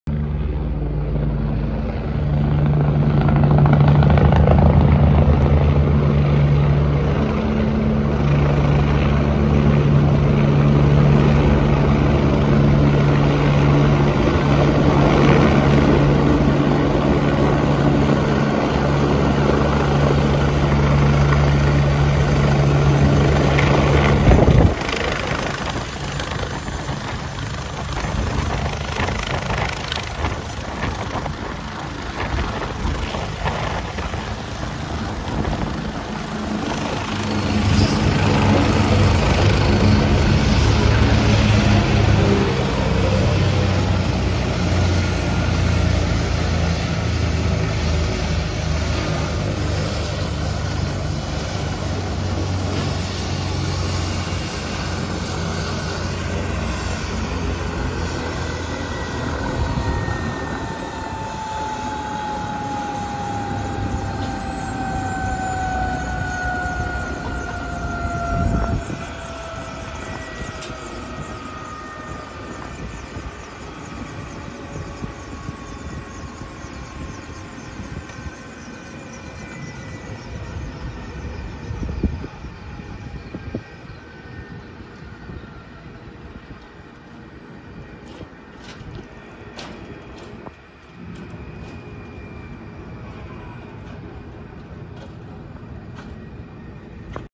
N901CM - LifeFlight of Maine AgustaWestland AW109E landing at CMMC - Sunday 6/5/2022, 11:17 AM
Video taken: Sunday, June 5, 2022 at 11:17 AM.